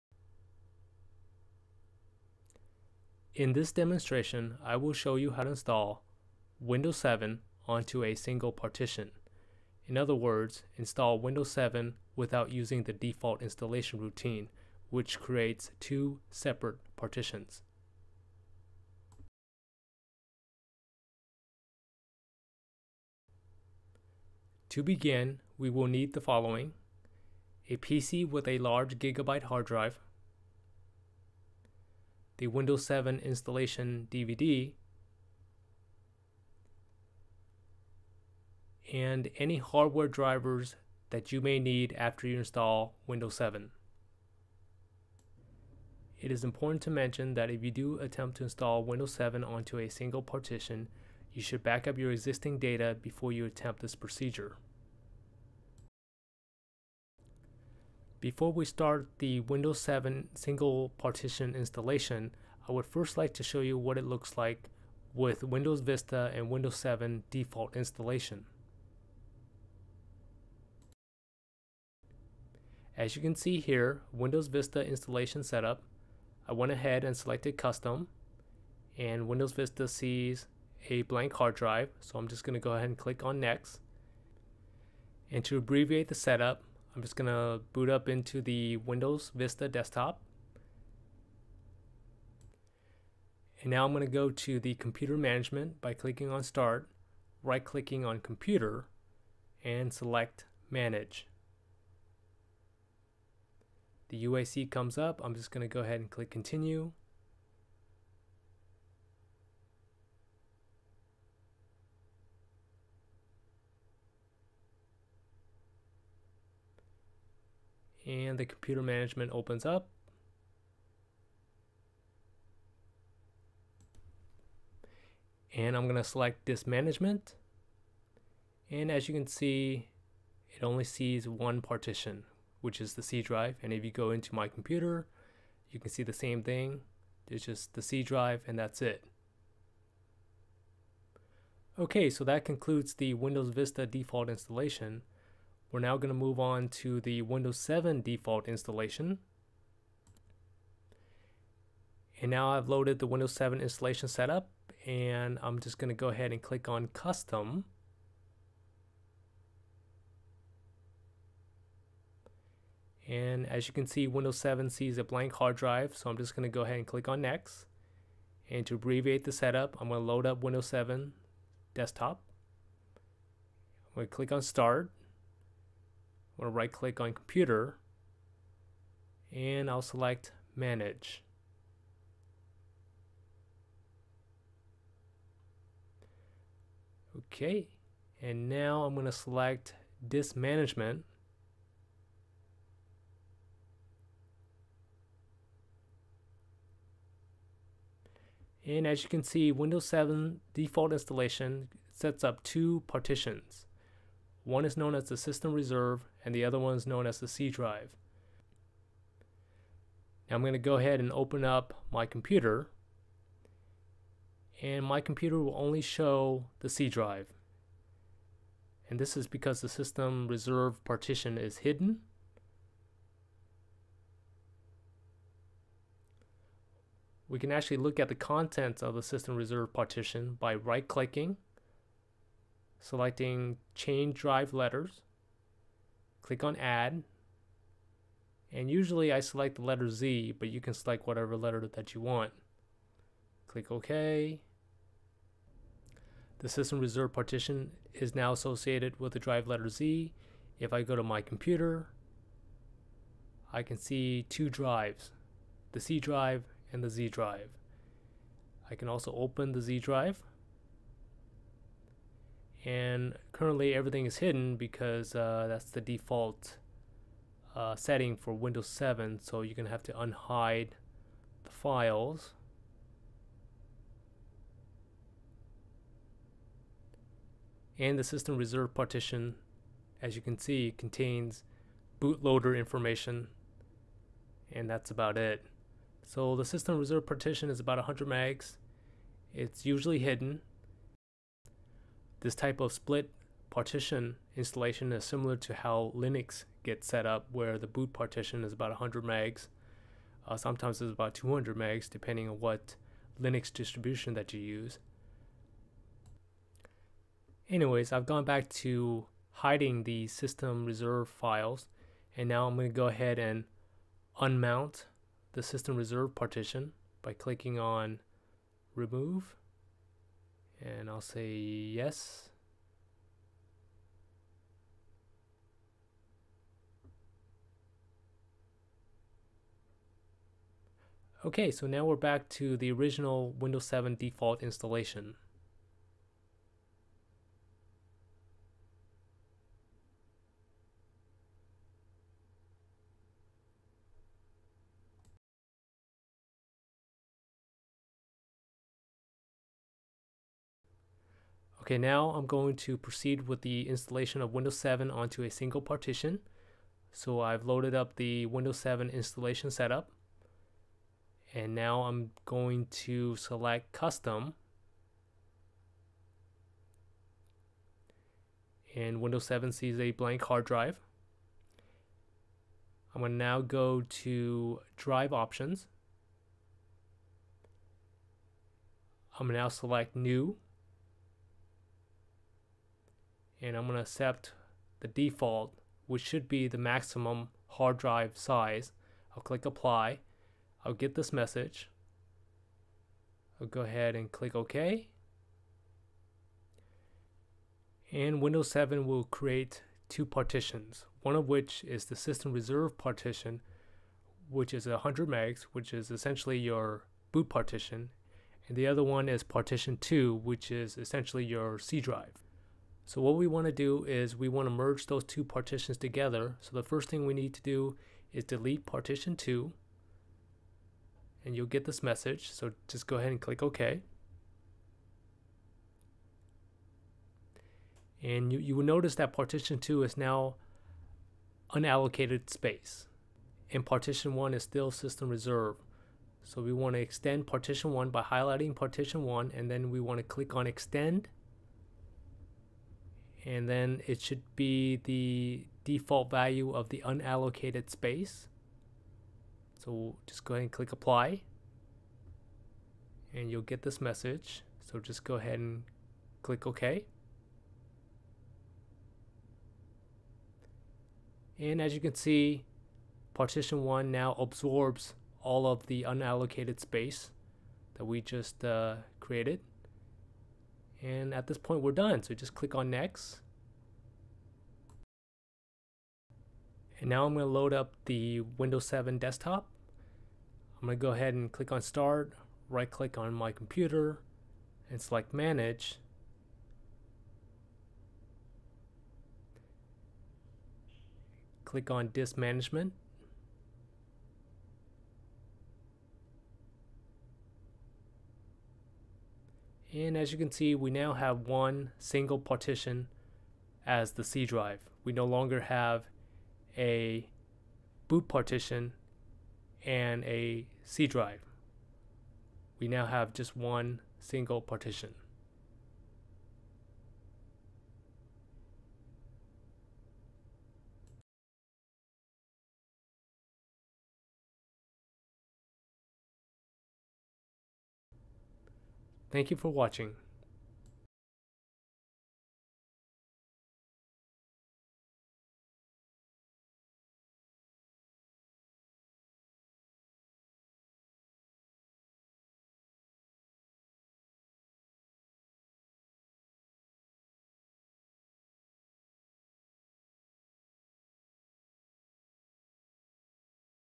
Install Microsoft Windows 7 onto a single partition
Windows 7 default installation creates 2 separate partitions:  System Reserved & Local Disk (C:).  This demonstration will show you how to install Windows 7 on a single partition without the extra System Reserved (Boot) partition.